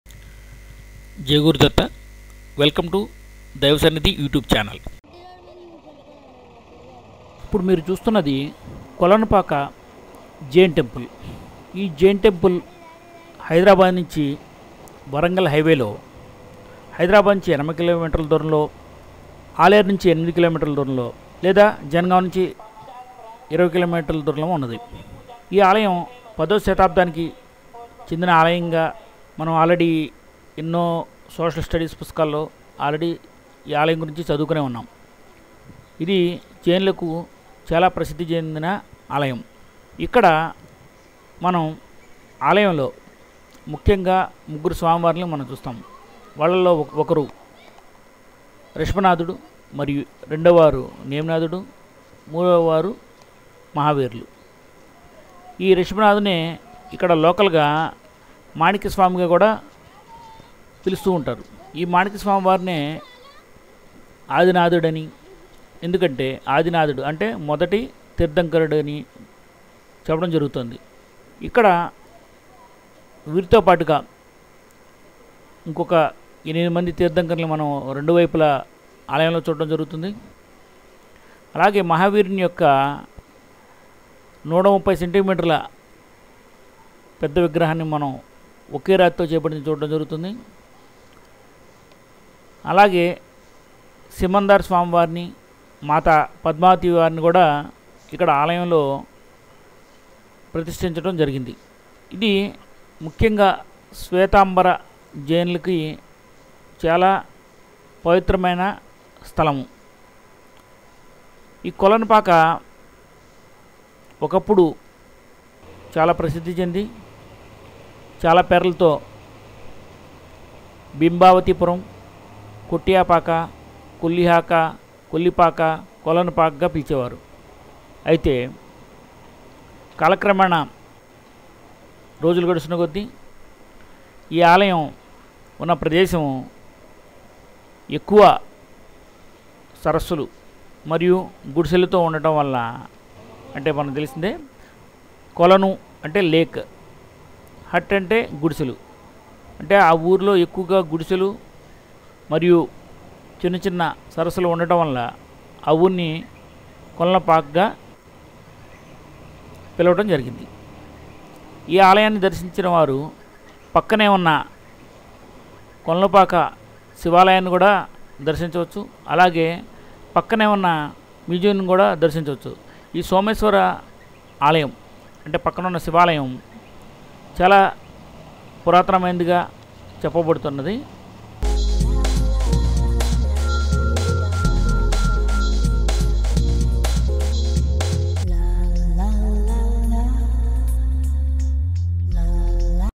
DAIVA SANNIDHI-Jain Temple, Kolanupaka, Nalgonda District-Kulpakji Temple
DAIVA SANNIDHI-Jain Temple, Kolanupaka, Nalgonda District

Kulpakji also Kolanupaka Temple is a 10th century Jain temple at the village of Kolanupaka in Nalgonda district, Telangana, India. The temple houses three idols: one each of Lord Rishabhanatha, Lord Neminatha, and Lord Mahavira. The image of Lord Rishabhanatha, carved of a green stone has been historically famous as "Manikyaswami". The temple is about 80 km from Hyderabad on the Hyderabad-Warangal Highway NH 163.  Lord Rishabha, popularly called Adinath Bhagvan, was the first Tirthankar in the Jain religion. It is believed that the original idol of Lord Adinath, known locally as Manikya Deva, has made Kolanupaka its abode.

There are eight idols of the other Tirthankars on both the sides of the main temple. The statue of Lord Mahaveer is 130 centimetres (51 in) tall and is said to be made of a single piece of jade. Idols of Lord Simandar Swami and Mata Padmavati are installed on either side of the main temple.

Kulpakji is an important pilgrimage center for Svetambara Jains of South India.

Also, the Someshwara Temple is very famous, which was established by Chalukya's about 800 years back. Kolanu means a Lake and Paka means a Hut. There used to be lots of lakes and huts and this caused to get this name. Kolanupaka is said to be known by different names in the past, Bimbavatipuram, Kottiyapaka, Kollihaka, Kollipaka and Kolanpak. Many statues were found while constructing the school and library in the village. All the statues were moved and placed in the Someshwara Temple's museum by Somalingam Kallem, a government official.